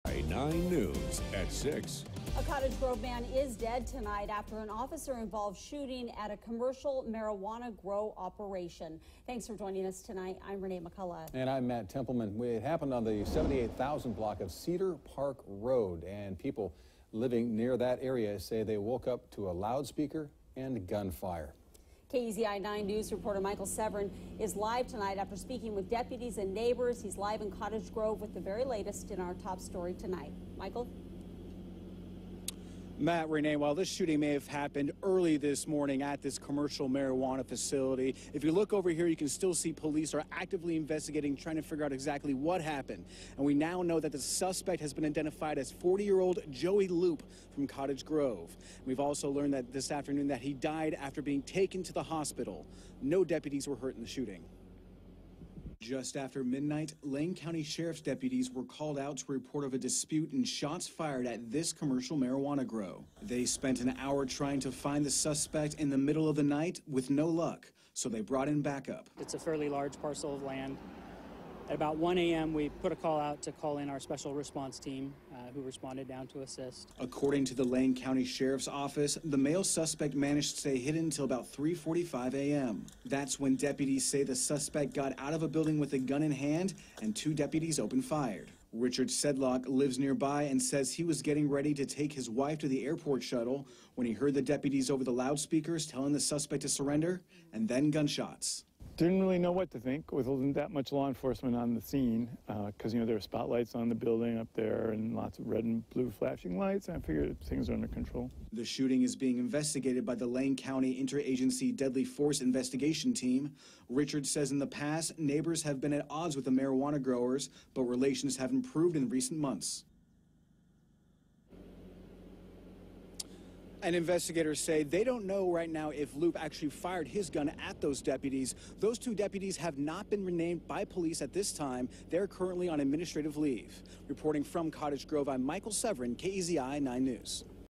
Man Dead After Officer-Involved Shooting In Cottage Grove
The man has been identified as Joey Loop, 40